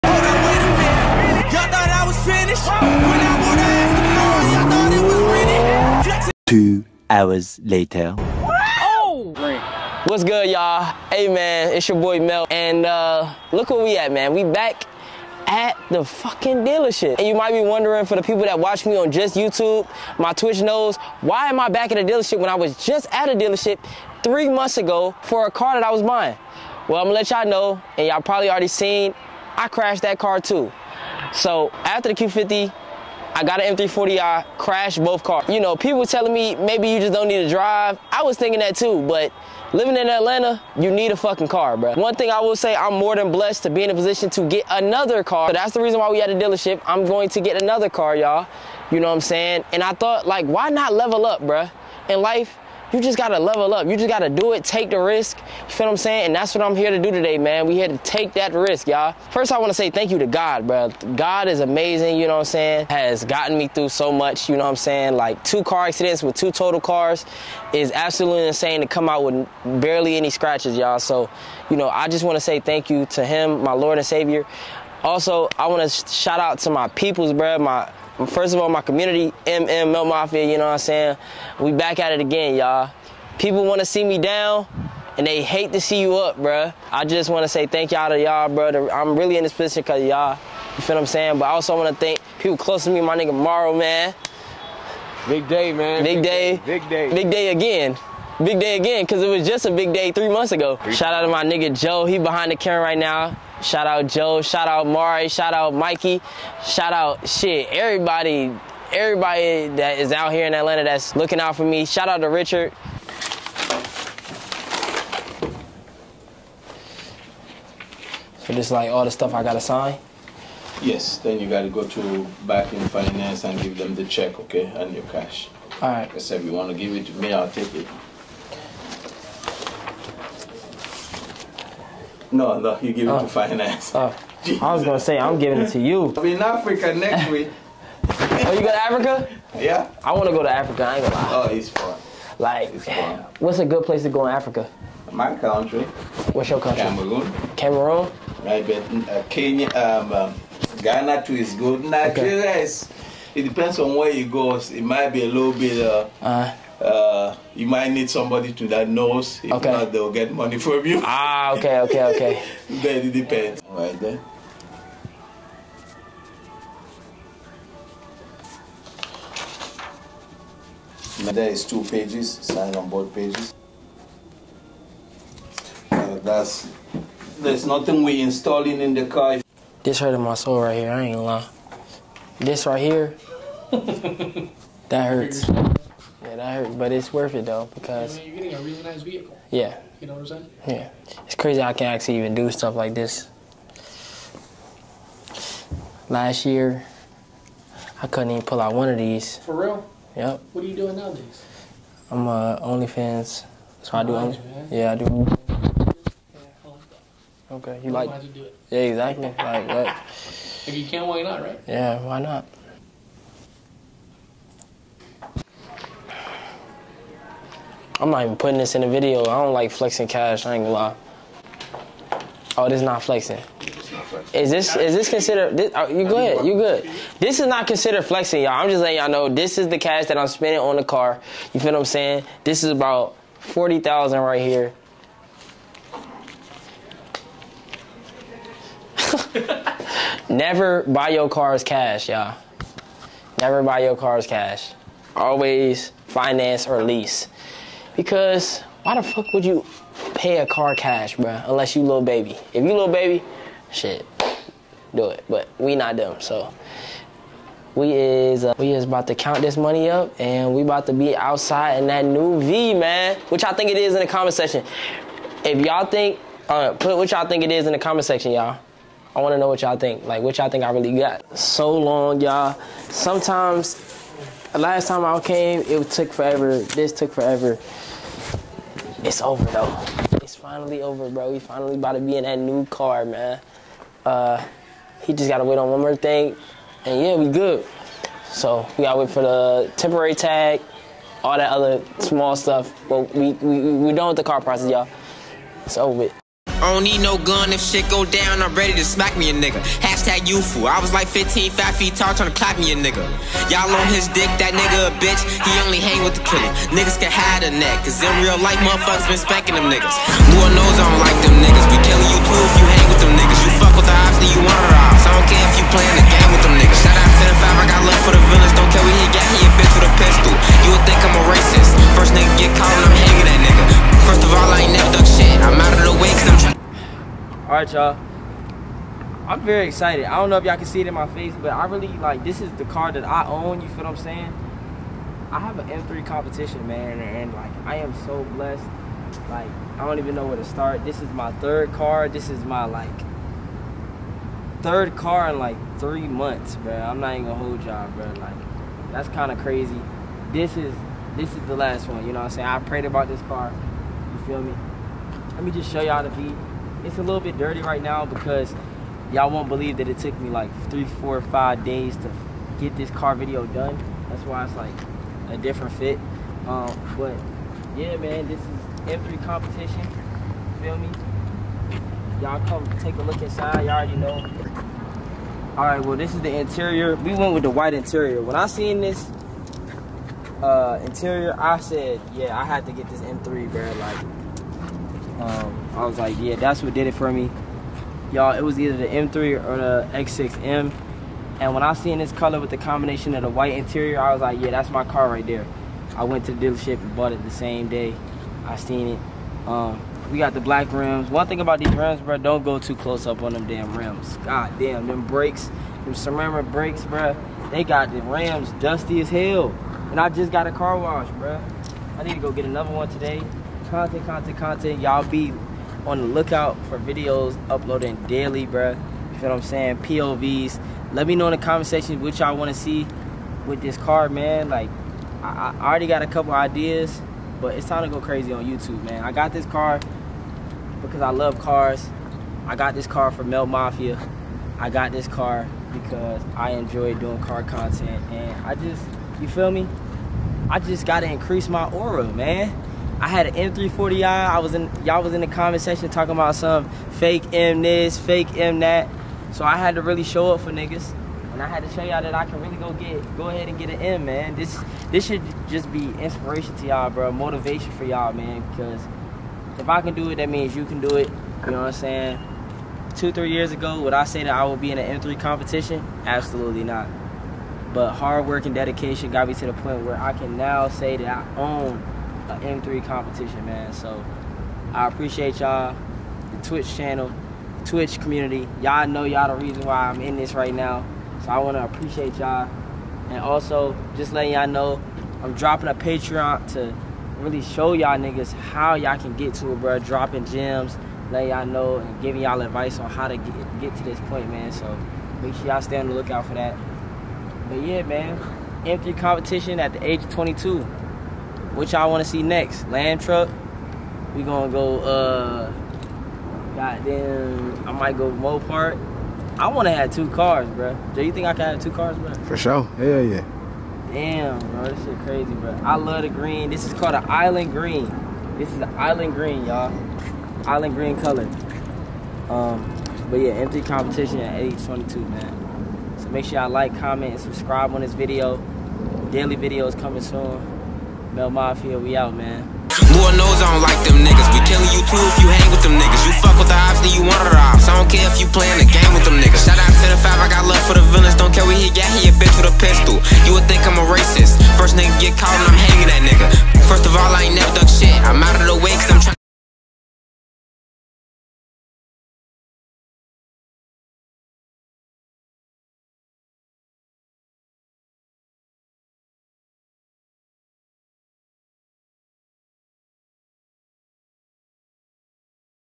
I BOUGHT A M3 COMPETITION AFTER CRASHING M340i AT 22 YEARS OLD!!
I bought my first bmw M3 competition

for more car content follow the twitch, I'm live everyday!